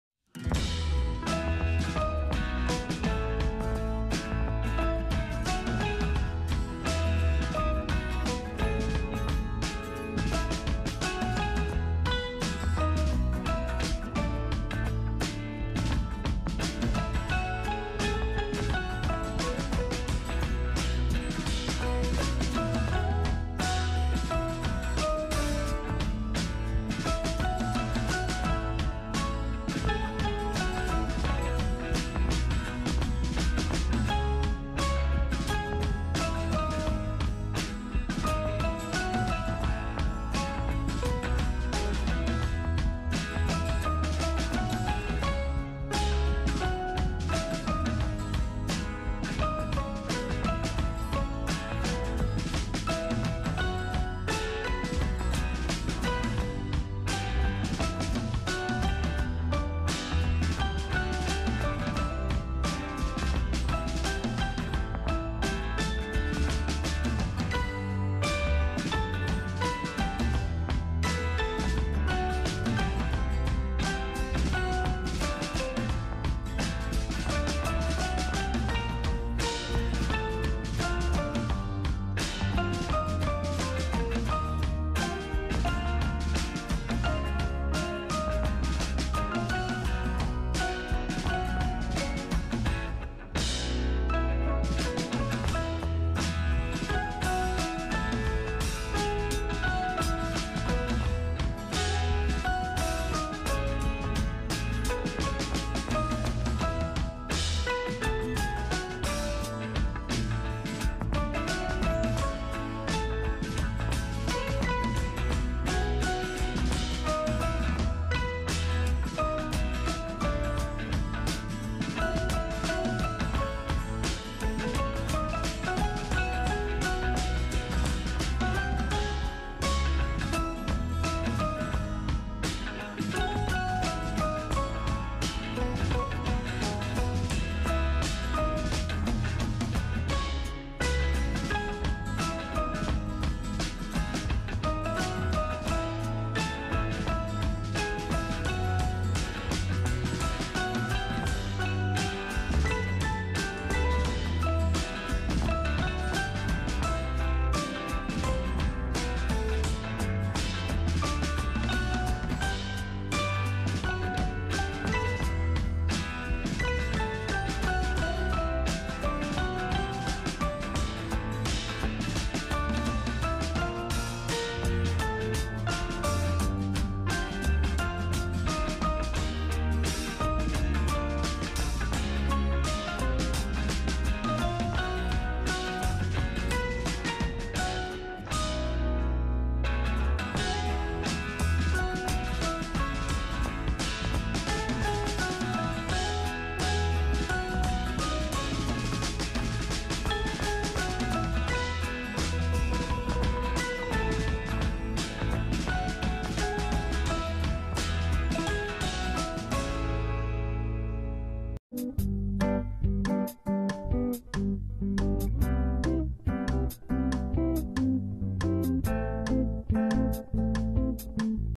P12.5M || House and lot for Sale in East Fairview Quezon City House Tour
House and lot for Sale in Fairview Quezon City

Floor Area;220

4 Bedroom
4 Toilet and Bath
2 Carpark

Total Price: 12.5M (nego)
30 Percent Down payment
70 Percent Thur bank
Rf: 200k